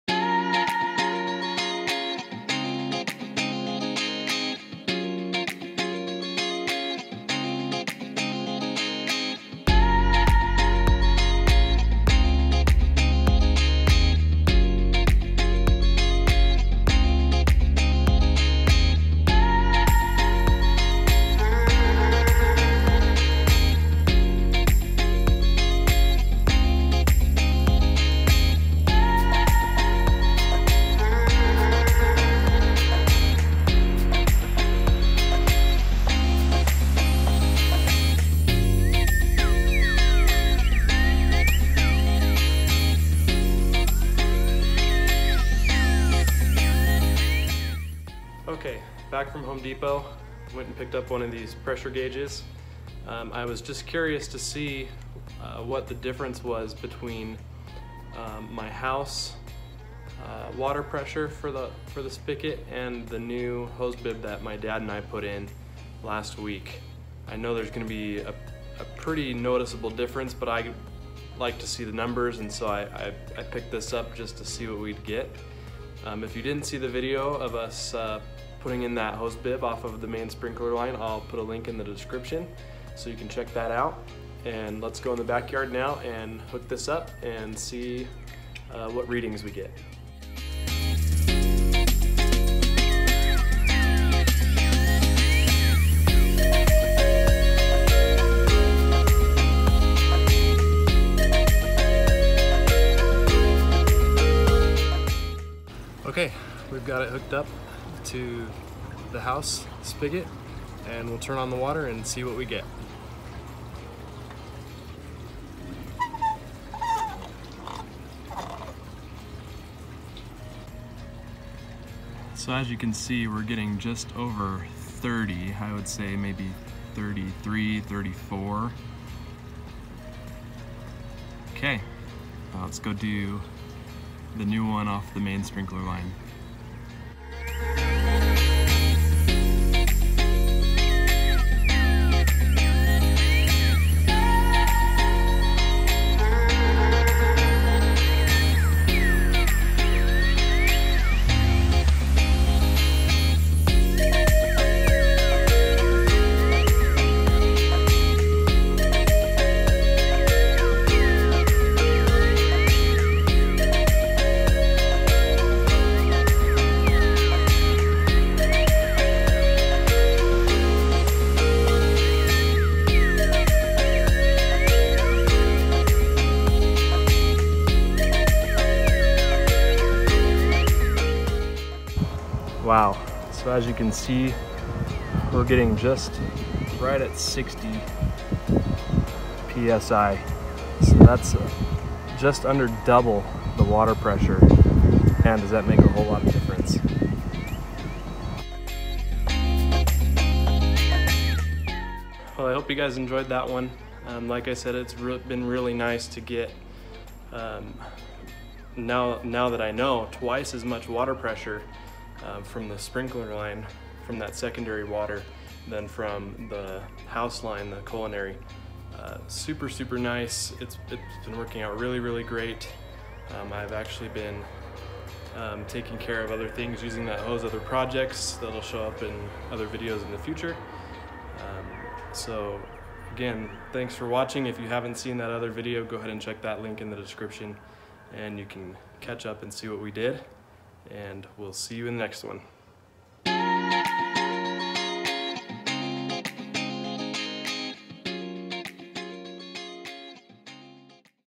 Hose Bibb Pressure Test
Last week my dad and I installed a hose bibb from the main sprinkler line in my backyard. This video shows my testing of the water pressure that I was getting from the culinary line at the house and comparing that to our new setup from the sprinkler line.